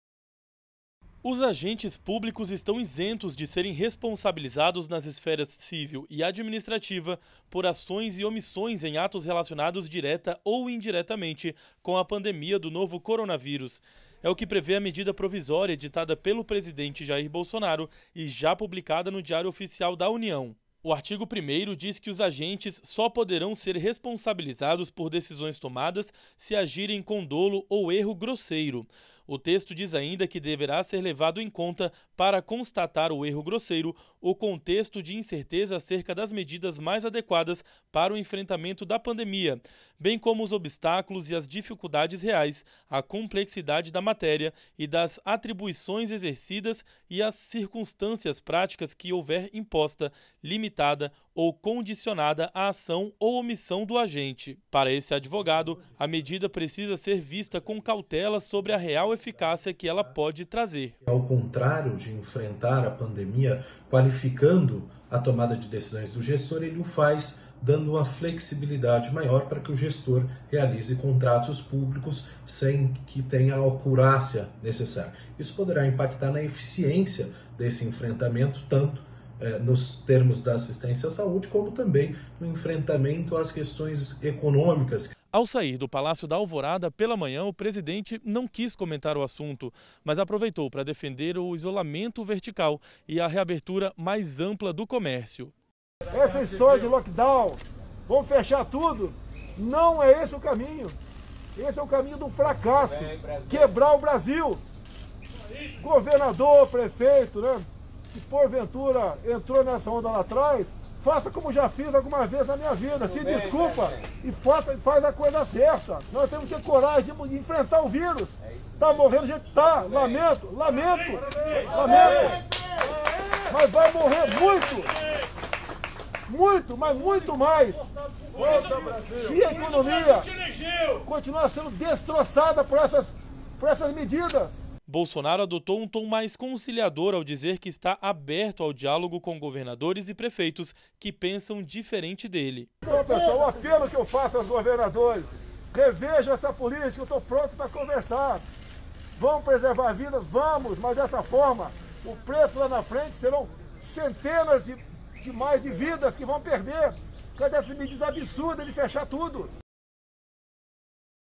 MP livra agente público de punição no enfrentamento da pandemia
Um decreto assinado pelo presidente Jair Bolsonaro pode beneficiar agentes públicos em eventuais erros e omissões em decisões durante a pandemia do novo coronavírus.

Reportagem de Filipy Barcelos e Bartolomeu Silva